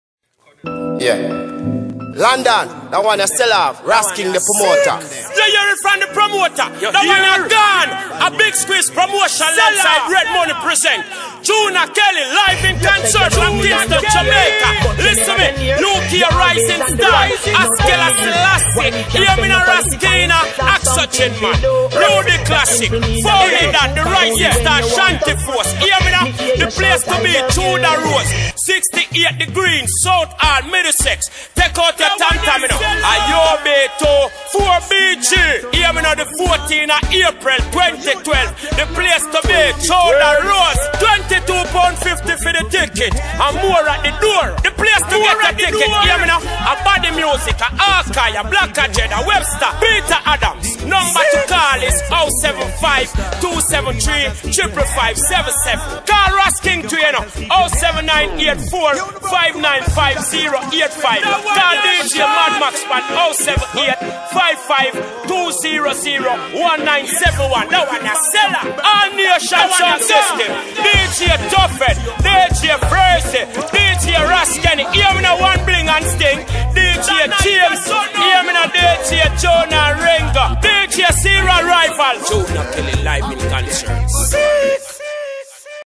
Junior Kelly LIve From Jamaica - Ras King Promotion
Ras King & Red Money Promotions Presents:
Live Performance From Junior Kelly - Fowlie Don - Ras Kinah - Oxygen Man &  Askala Selassie .
@ Tundor Rose 68 The Green South Hall UB2 4BG
Tickets 22.50